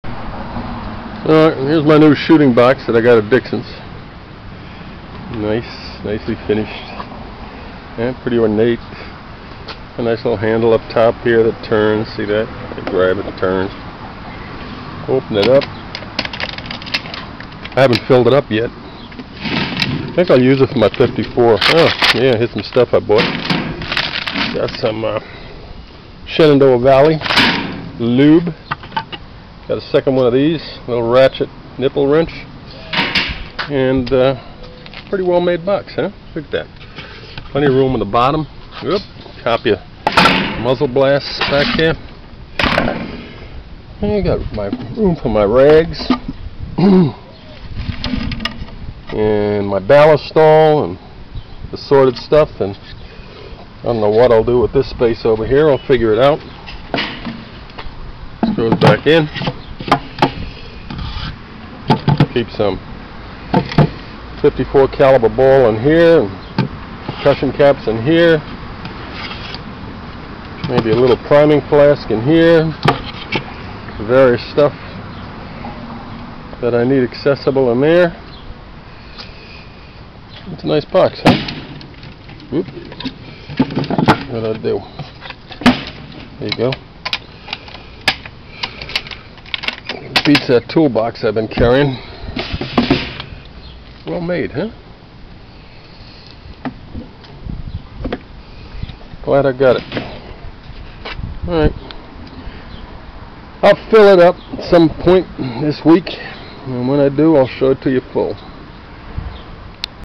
ShootingBox
My new muzzleloader shooting box. Maybe I'll dedicate it to 54 caliber. That's what I usually shoot. But maybe I'll make it a general shooting box for all calibers. We'll see.